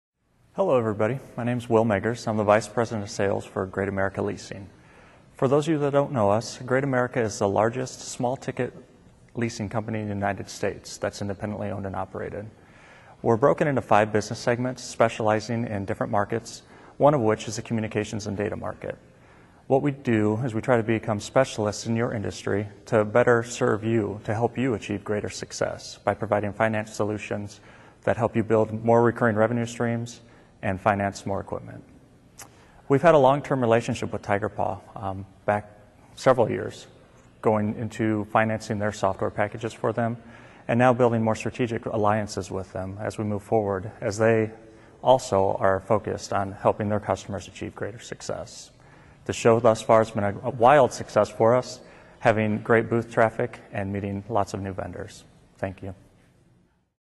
Tigerpaw User Conference 2010 - Great America Leasing
Wil Meggers, Vice President of Sales at Great America Leasing talks on camera at the 2010 Tigerpaw User Conference